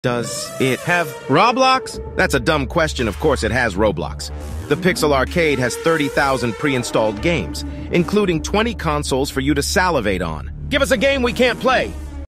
Does it have roblox?
Does the pixel arcade have roblox?

find out now in today's short video.

Also roblox is kids game